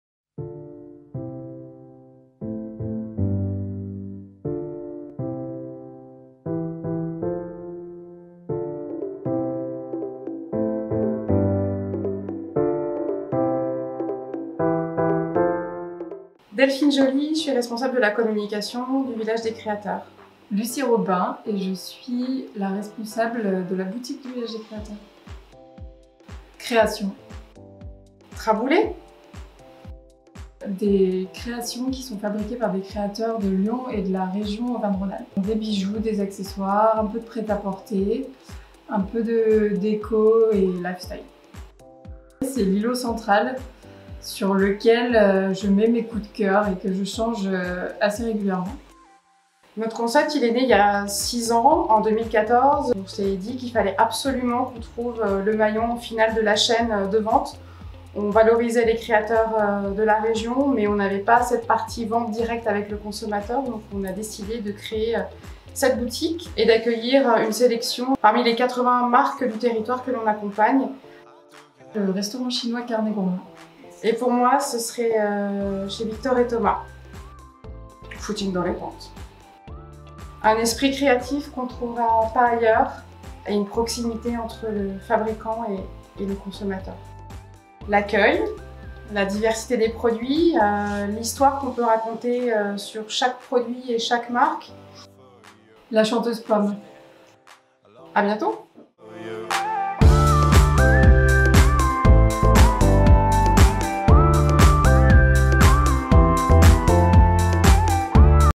L’INVITE(E) MY PRESQU’ILE - Lucie & Delphine, Village des Créateurs
✨ L’INVITE(E) MY PRESQU’ILE 🎬

Aujourd’hui nous rencontrons Lucie et Delphine du Village des Créateurs. Située dans le beau passage Thiaffait, on y trouve de tout : de la mode, de la déco, des accessoires pour enfants, du lifestyle…

Tous ces produits sont fabriqués par des créateurs de Lyon et de la région Auvergne-Rhône-Alpes.

Lucie saura vous conseiller et vous raconter l’histoire des produits qu’elle présente !

A bientôt chez le Village des Créateurs
📍 Passage Thiaffait 19 rue René Leynaud 69001 Lyon

Plus d'actualités sur les boutiques du centre-ville de Lyon, rendez vous Facebook et Instagram @mypresquile

vidéo filmée et montée par Coline Thomas